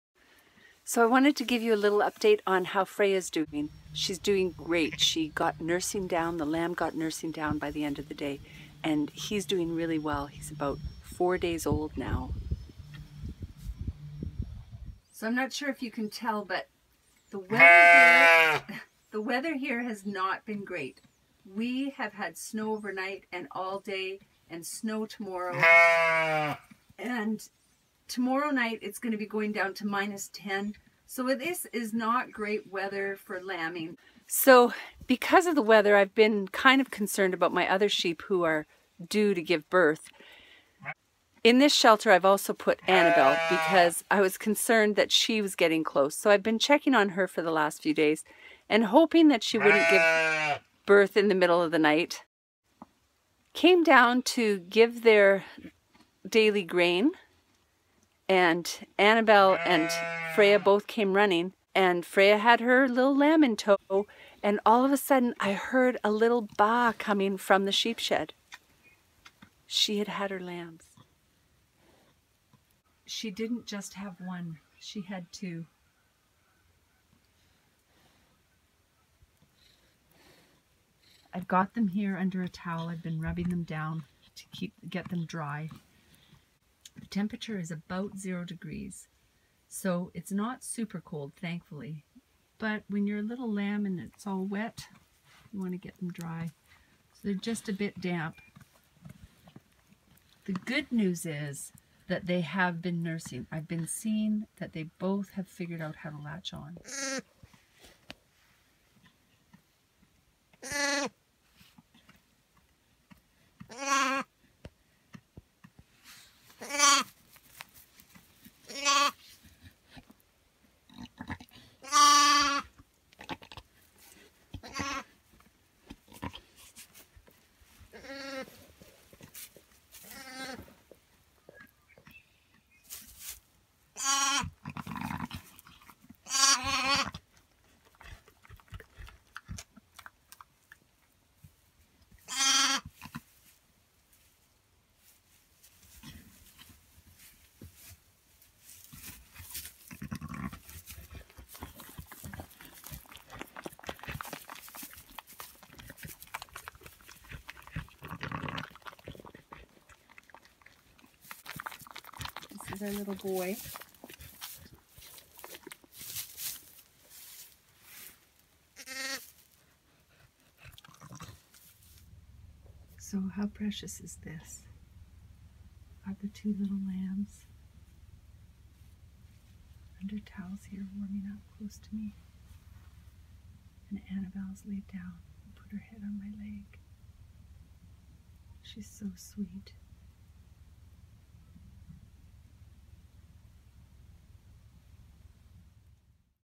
Update on Freya
Freya was the first of our sheep to give birth in 2017, but others follow.